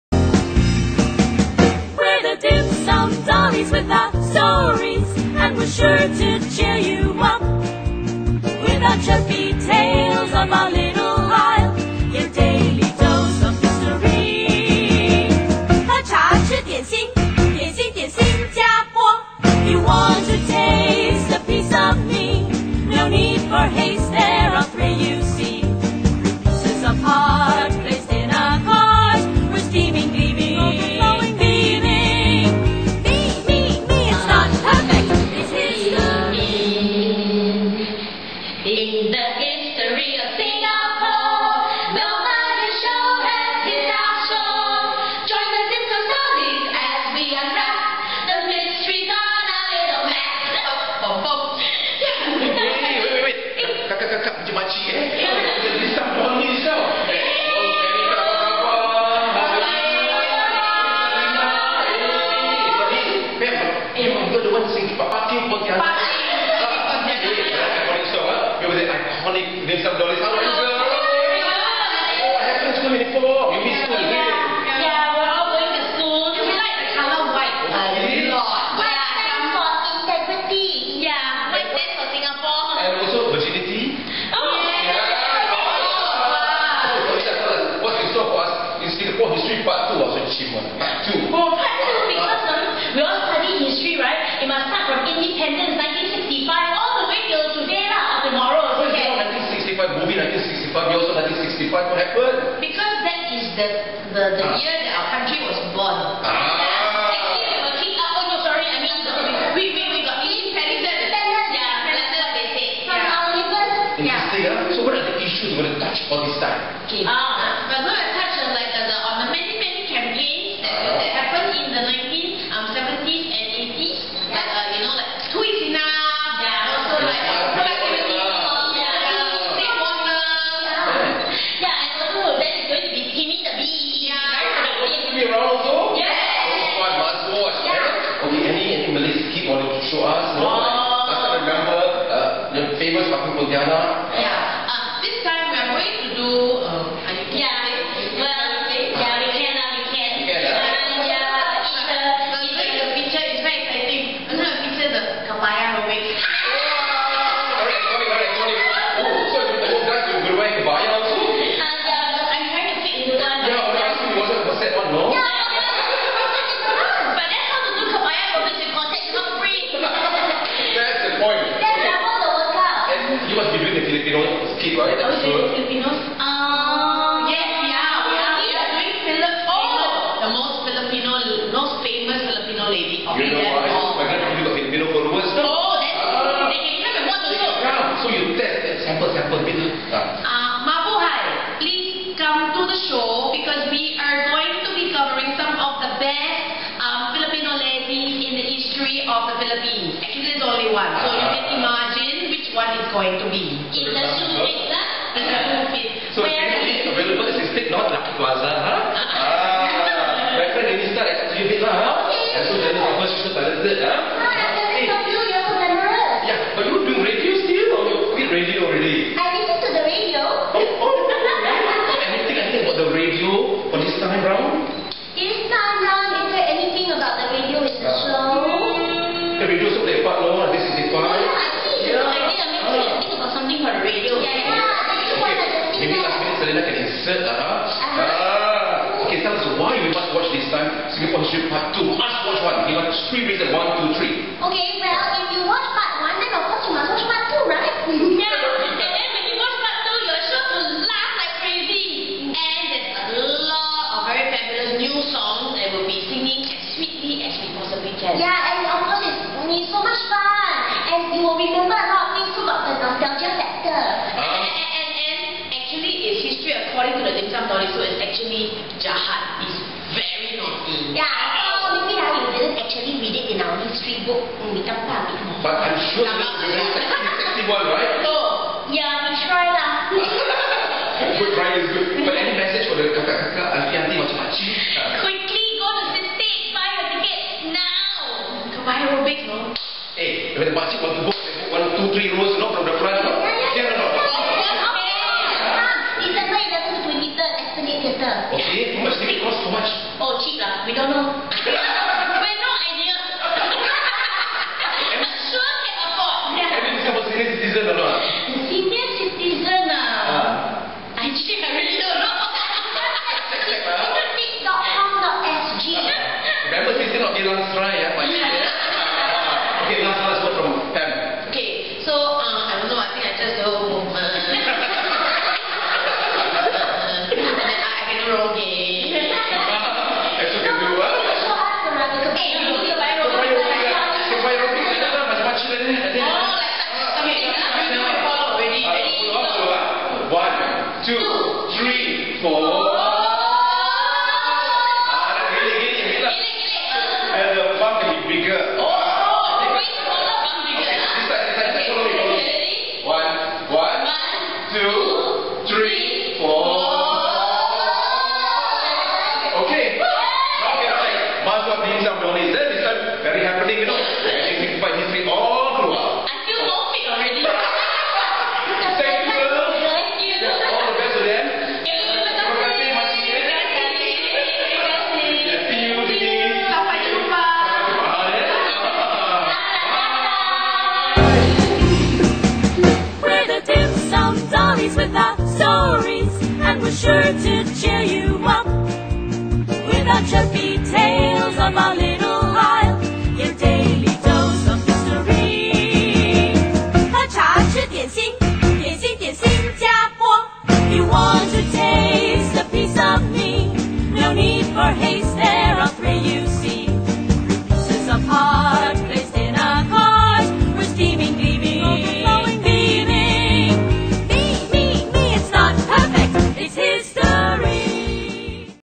Glam TV: Exclusive DJ Hafeez Glamour with Dim Sum Dollies
Exclusive DJ Hafeez Glamour with Dim Sum Dollies on their  upcoming brand new show “The History Of Singapore, Part 2 The Growing Years”
Saturday 1st Nov 2014. Enjoy !